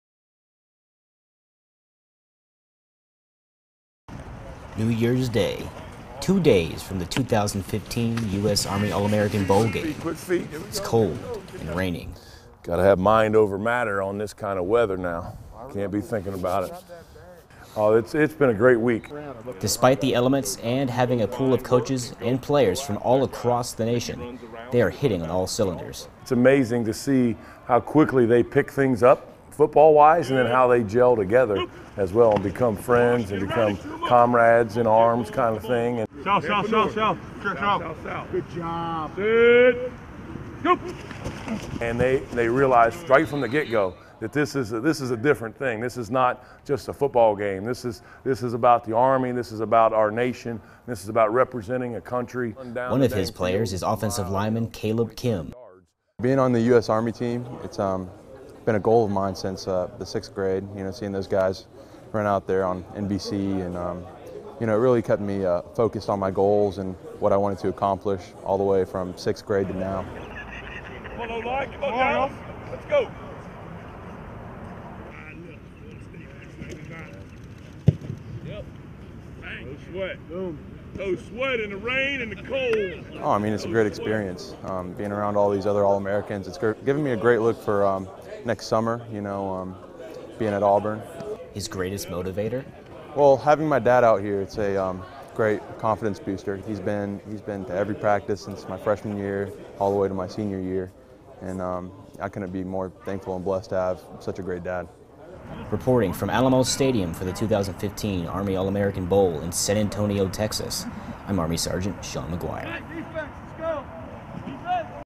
Team East Practice
Players and coaches for Team East practice ahead of the 2015 Army All American Bowl game in San Antonio, Texas. Shot and produced by SGT Sean McGuire, US Army Reserves.